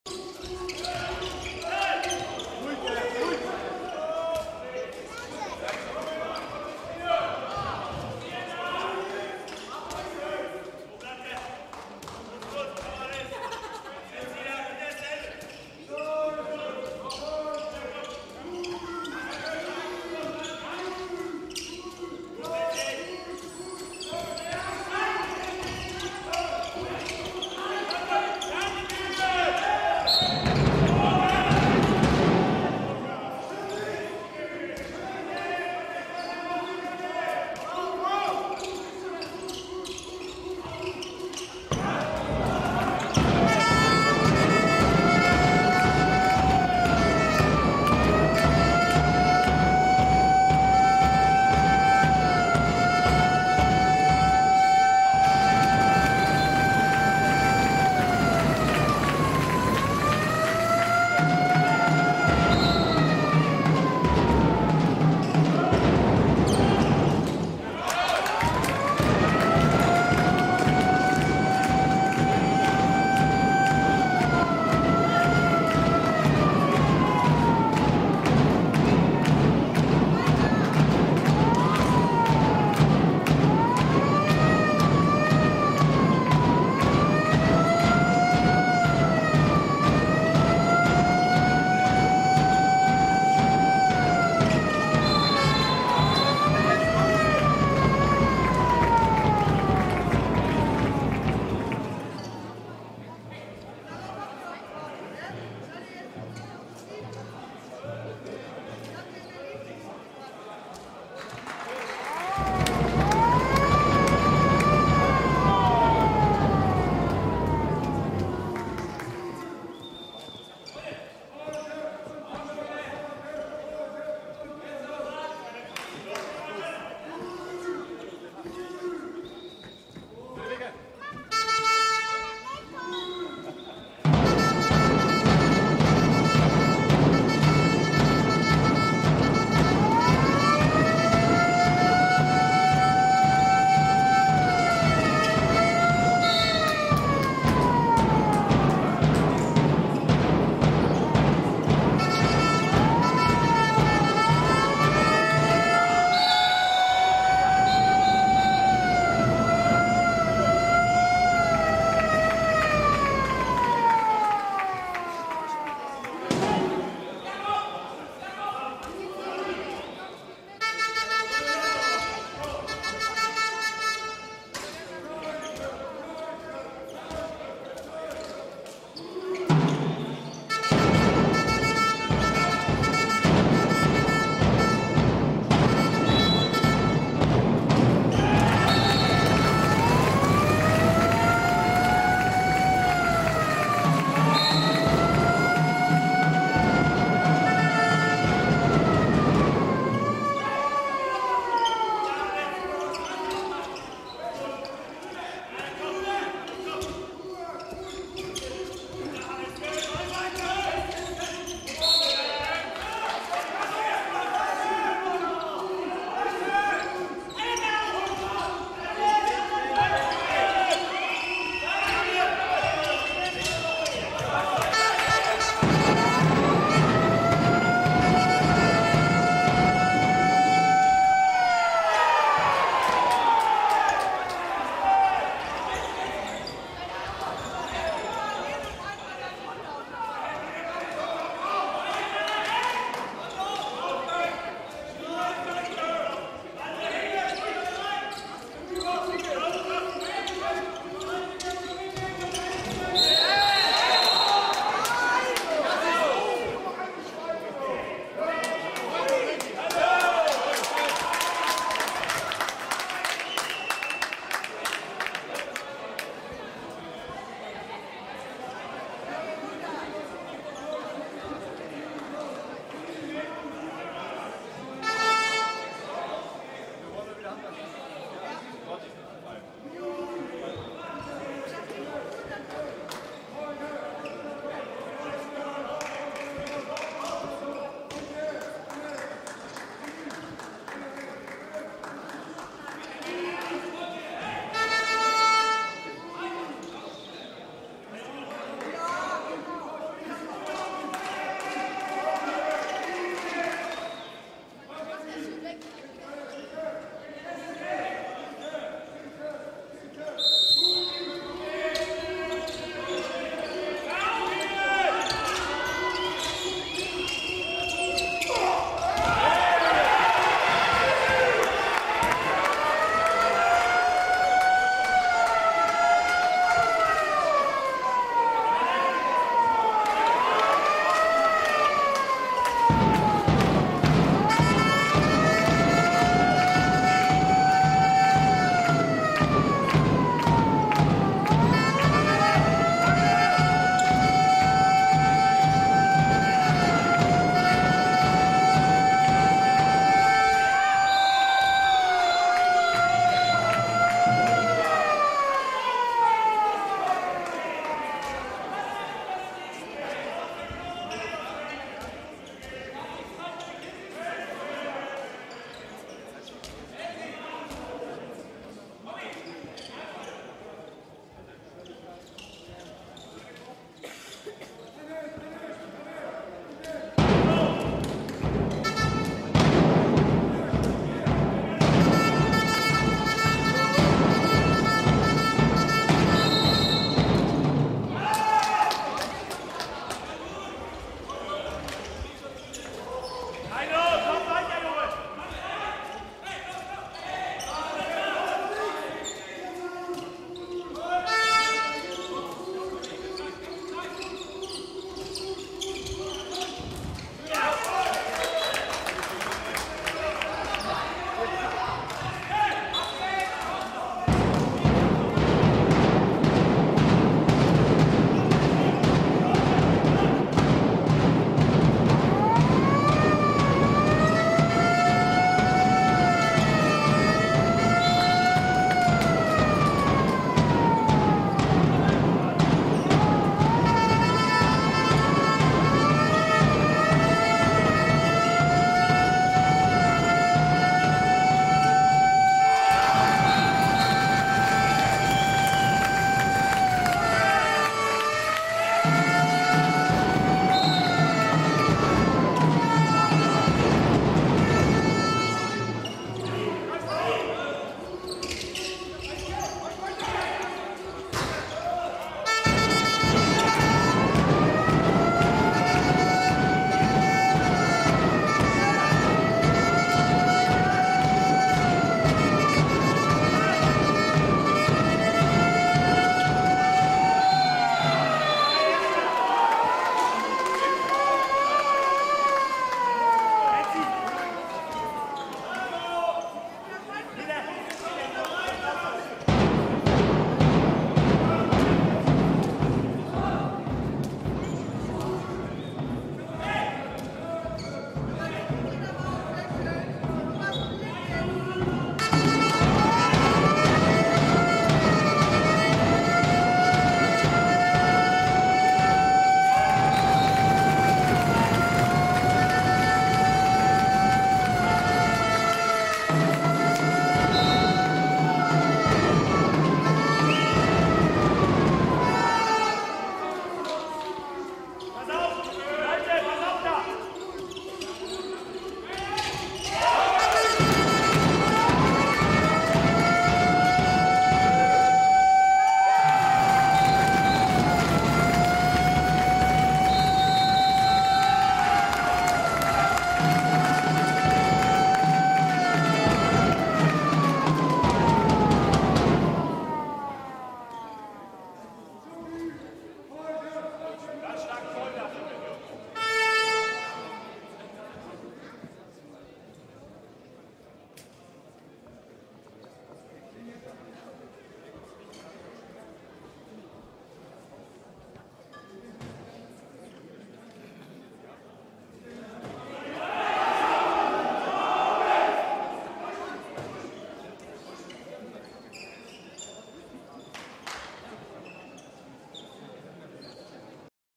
Sport inSchwerin 23s2 T 07
Handball Männer Regionalliga SV Post Telekom Schwerin II vs. THW Kiel II